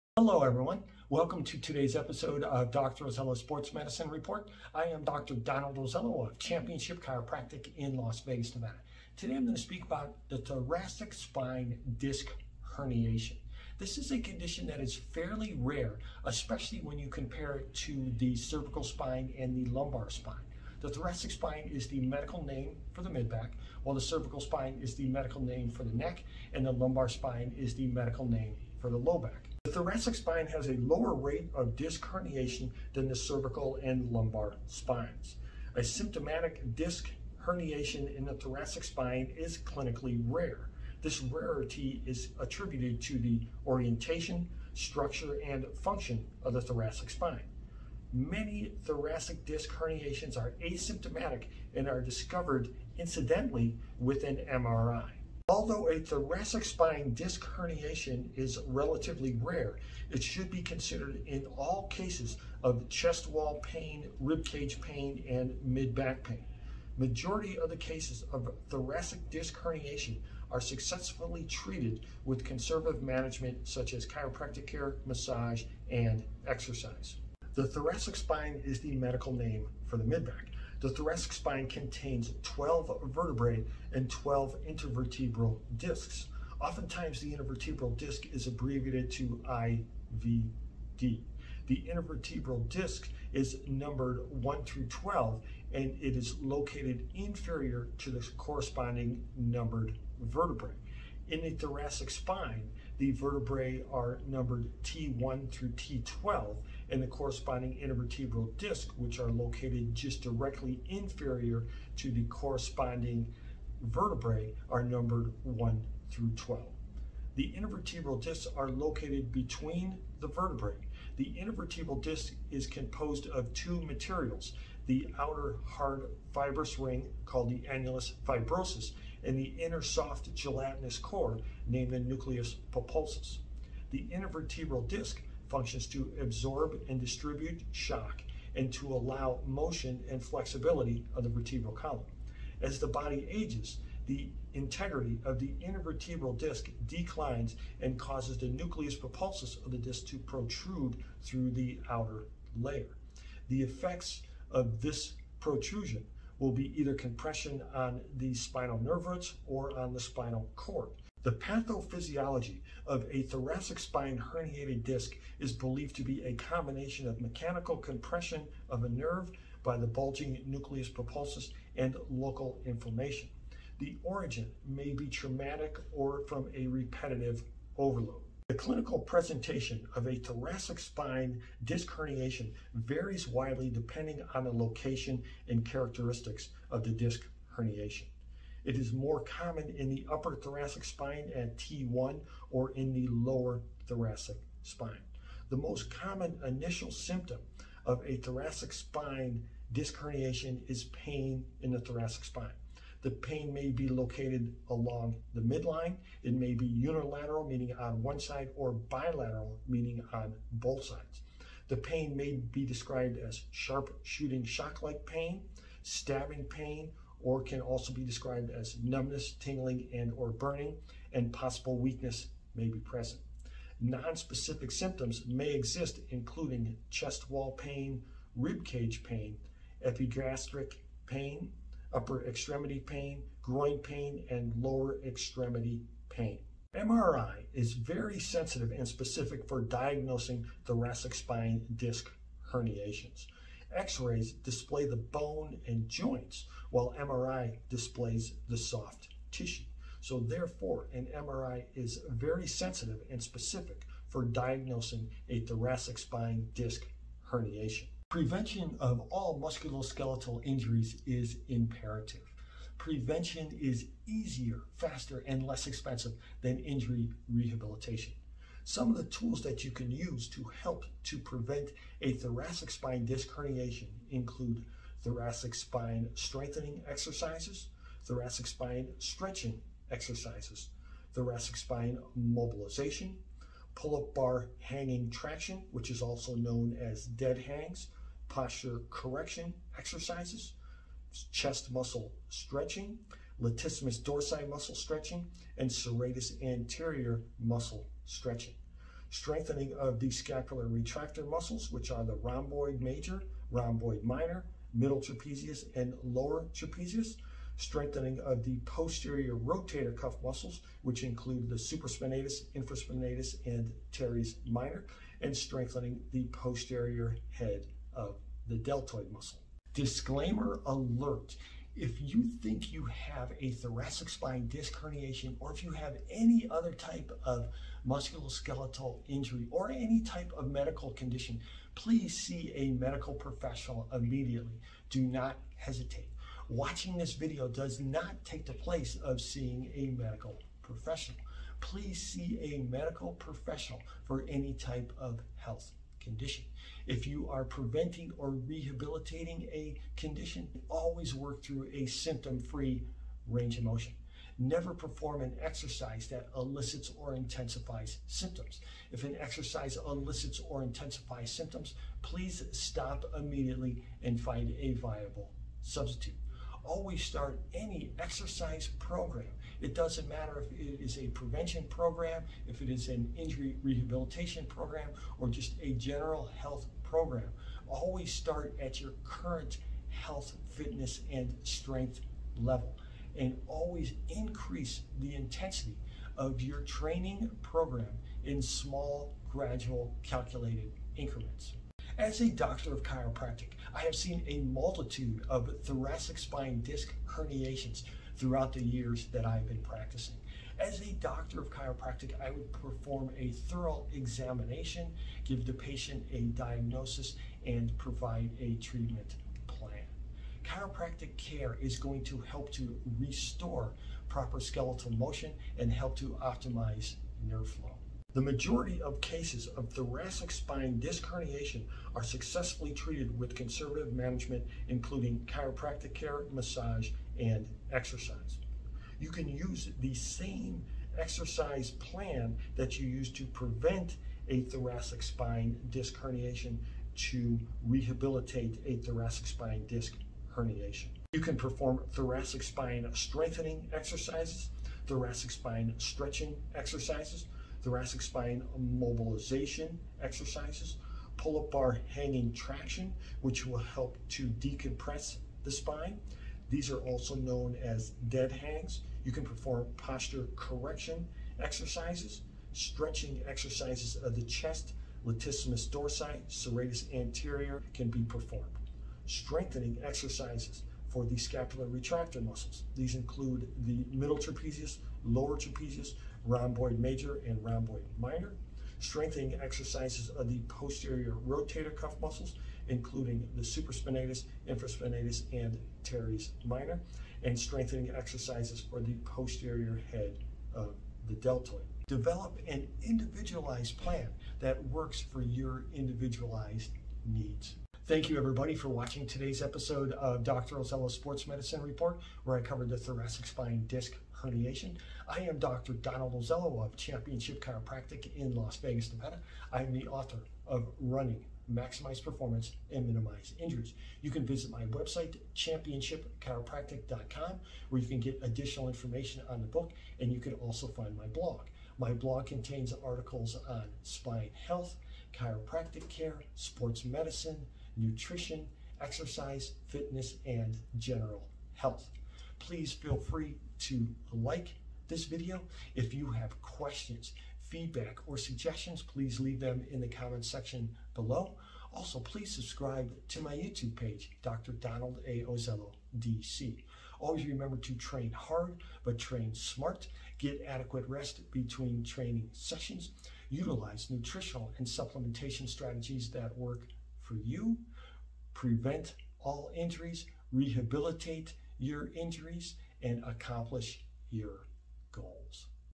Thoracic Spine Disc Herniation
Dr Ozello's Sports Medicine Report: Thoracic Spine Disc Herniation and Thoracic Spine Disc Bulge

Serious pathology must be ruled out first in all chest and rib cage pain.

Thoracic spine has a lower rate of disc herniation than cervical and lumbar spine. Symptomatic thoracic disc herniation is clinically rare. This rarity is attributed to the orientation, structure and function of the thoracic spine. Many thoracic disc herniations are asymptomatic and discovered incidentally with an MRI

Anatomy: Intervertebral discs are located between the vertebrae. The intervertebral disc is composed of two materials: the outer hard fibrous ring called the annulus fibrosis, and an inner soft gelatinous core named the nucleus pulposus. The intervertebral disc functions to both absorb shock and allow flexibility of the vertebral column. As the body ages, the integrity of the intervertebral disc declines and causes the inner core of the disc to protrude through the outer layer. The effects of these will be either compression of the nerve roots or the spinal cord, giving rise to radicular symptoms.

Pathophysiology of herniated discs is believed to be a combination of mechanical compression of a nerve by the bulging nucleus pulposus and local inflammation.
Origin may be trauma or repetitive overload.

Clinical presentation varies widely depending on location and characteristics.
More common in upper thoracic spine at T1 or in the lower thoracic spine.
Most common initial symptom is thoracic pain.
Pain can be midline, unilateral or bilateral.
Sharp shooting shock-like pain. Numbness, tingling and/or burning. Possible weakness.
Nonspecific symptoms may exist including chest wall pain, epigastric pain, upper extremity pain, groin pain and lower extremity pain.

Imaging: X-rays display the bone and joint while MRI displays the soft tissue. MRI is very sensitive and specific for diagnosing thoracic spine disc herniation.

Majority of cases of thoracic disc herniation are successfully treated with conservative management.
Though relatively rare, thoracic disc herniation should be considered in cases of chest wall pain and midback pain.

Prevention: Thoracic spine strengthening, thoracic spine stretching, thoracic spine mobilization, pull-up bar hanging traction (Dead Hangs), posture correction exercises, stretching of chest, latissimus dorsi and serratus anterior muscles and strengthening of scapular retractor muscles (Rhomboids, middle traps and lower traps), posterior rotator cuff muscles (Supraspinatus, Infraspinatus and Teres Minor) and posterior head of deltoid.

Chiropractic Care: The primary objectives of chiropractic treatment are the optimization of nerve flow and the re-establishment of correct skeletal biomechanics.

Thoracic Spine T1-T12
Each thoracic vertebra articulates with ribs bilaterally.
Twelve pairs of thoracic nerve roots. Twelve Intervertebral Discs (IVD). Located below the corresponding numbered vertebra.

Intervertebral Disc increases spinal motion and provides the majority of the spine’s weight-bearing capacity.
Function: Provides a measure of shock absorbing protection to the spinal column and appropriate stability for the spine during load bearing activities.
The inferior surface of the superior vertebral body articulates with the superior surface of the inferior vertebral body through intervertebral (IV) discs.
Located between each of the individual vertebra except C1 and the skull and C1 and C2.